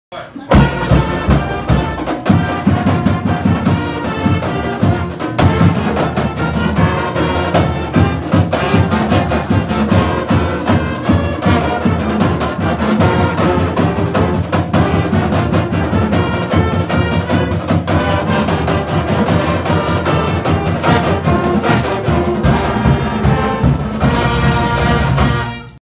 Fight song
Band is hard at practice this afternoon. Dontcha just love our fight song? Play it LOUD!